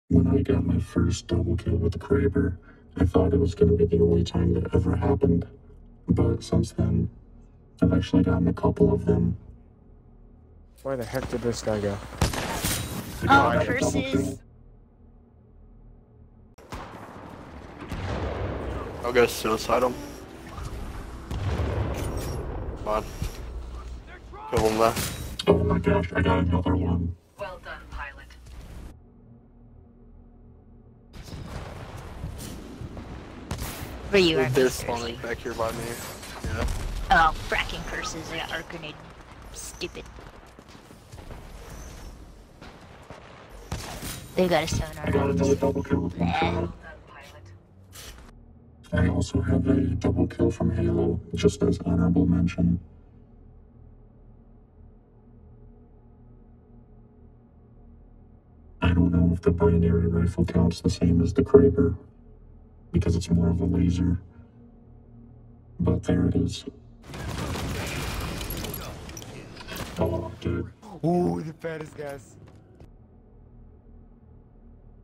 Sniper double kills
I think I have 7 with the Kraber, never got to record the first because I didn't know how to record on xbox.